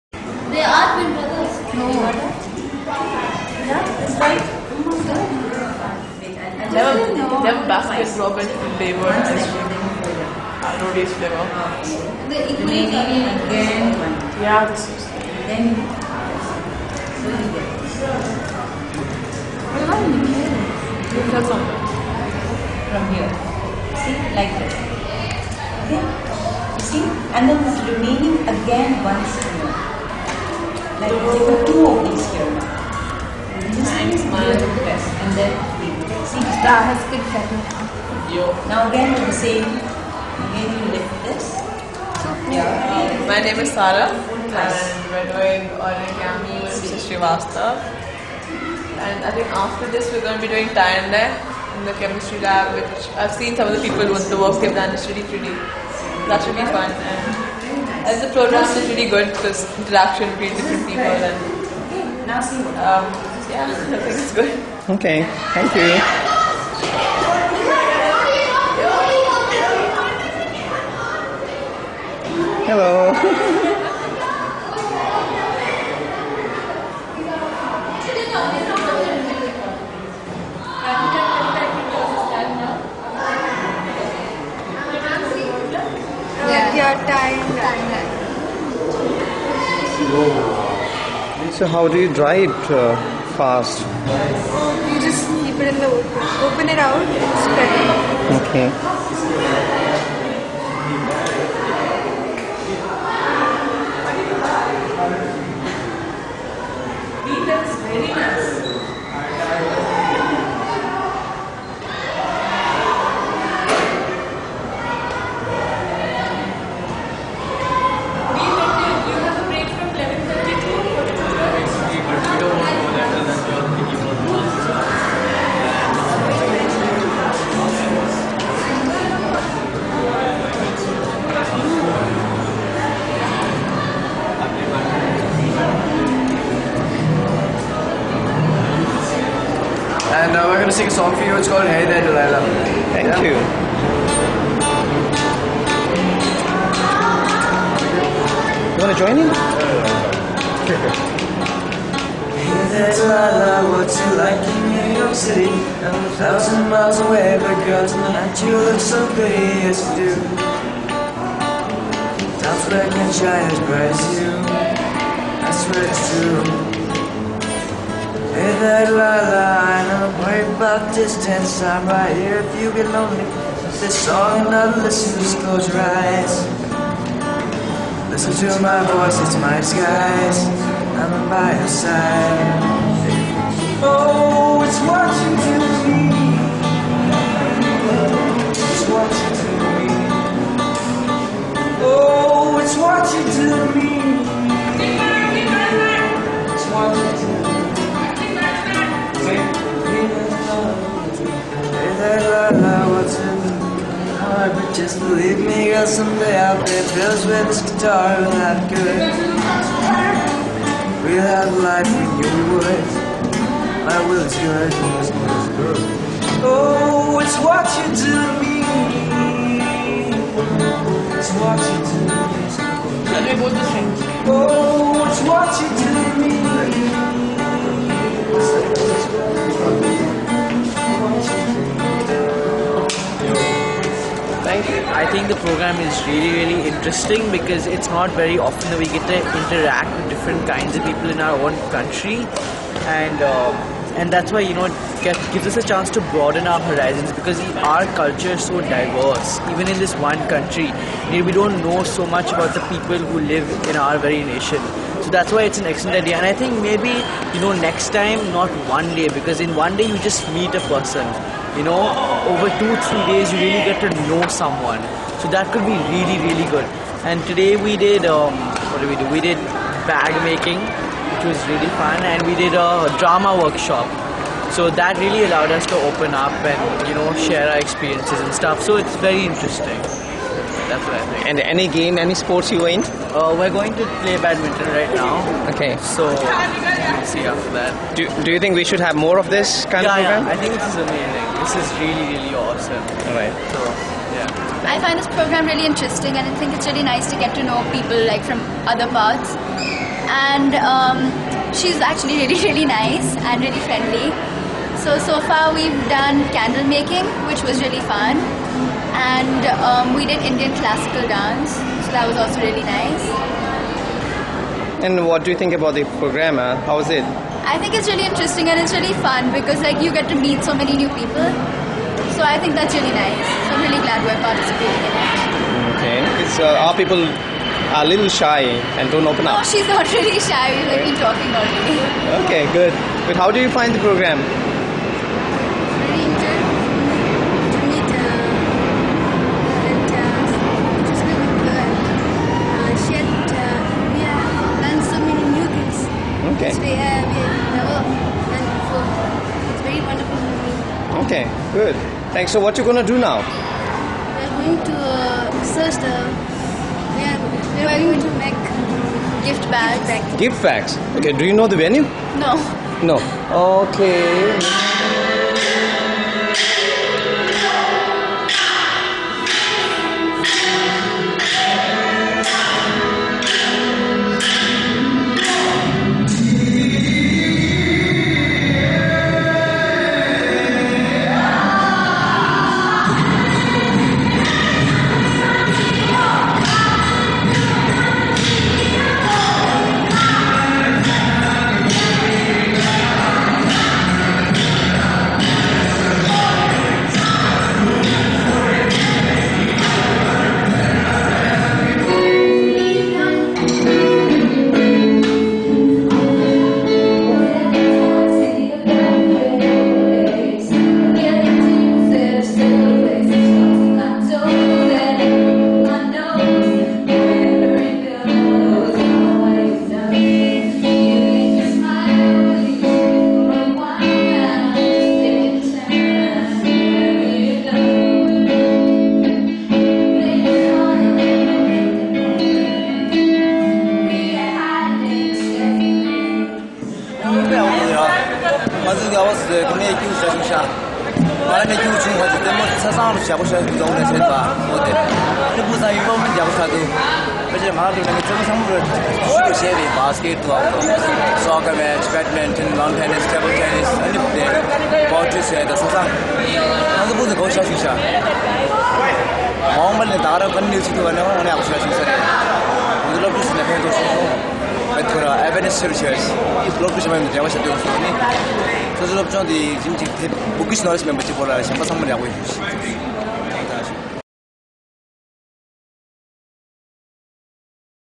Opportunity Tour 2008 - Part 3.flv
In Dec 2008, ENVISION facilitated Opportunity Tour for the students of Tibetan Homes Foundatin School, Mussoorie. The entire tour was funded by THF school itself except for ENVISION staff members. One day programme at Vasant Valley School (VVS), New Delhi was conceptualised by the ENVISION and VVS staffs together and facilitated.